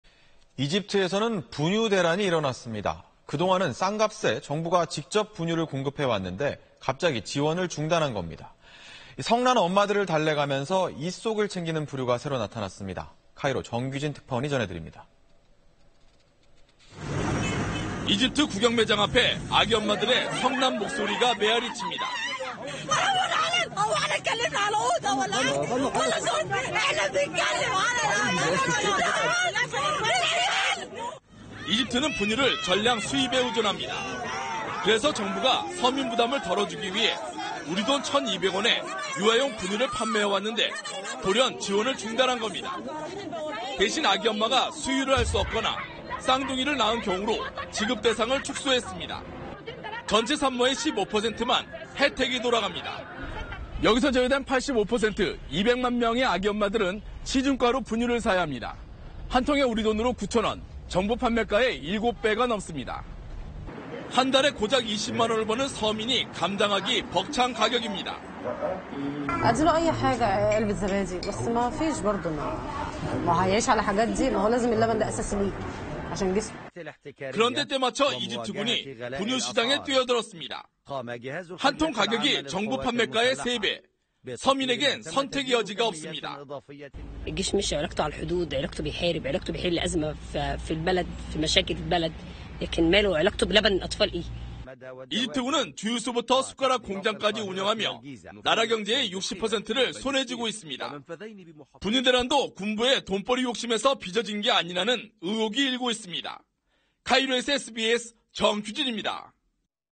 나라 경제 60% 쥔 이집트 군…이번엔 분유 장사/SBS
이집트에서는 분유 대란이 일어났습니다. 그동안은 싼값에 정부가 직접 분유를 공급해 왔는데, 갑자기 지원을 중단한 겁니다. 성난 엄마들을 달래가면서 잇속을 챙기는 부류가 새로 나타났습니다.

카이로 정규진 특파원이 전해드립니다.